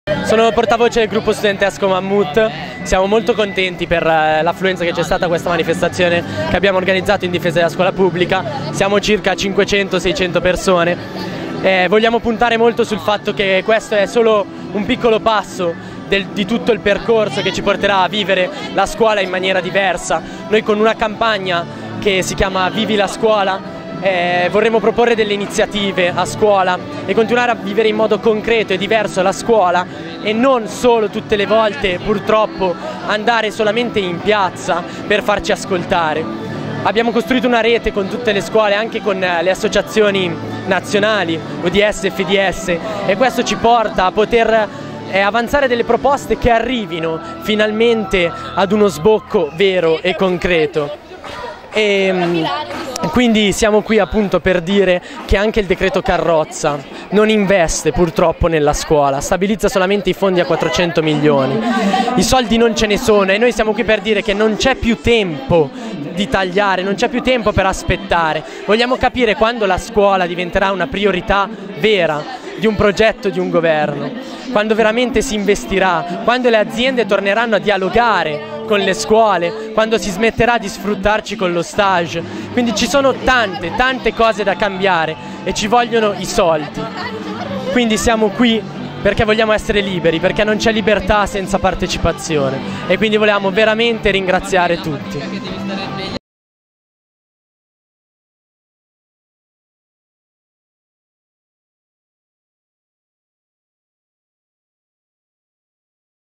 Manifestazione studentesca di Arona - 11/10/'13
Abbiamo seguito il corteo studentesco di Arona in difesa della scuola pubblica. Intervista al portavoce del gruppo studentesco Mammuth